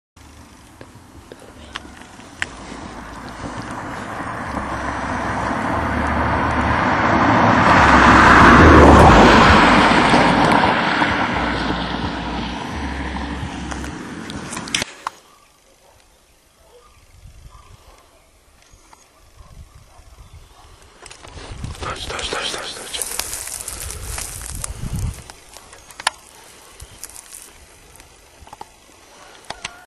Gusterul (Lacerta viridis)
Gusterul, soparla de smarald sau soparla verde este cea mai mare si cea mai frumos colorata soparla care populeaza Germania, ajungand pana la 40 cm. sau chiar 50 cm., cum este cazul raselor sudice. De obicei, masculul depasteste cu cativa centimetri in lungime femela. Sunt animale puternice, cu statura zvelta, pe care le-o confera coada lunga, reprezantand doua treimi din marimea totala a animalului. Coada are un rol important si in deplasarea animalului, dandu-i rapiditate.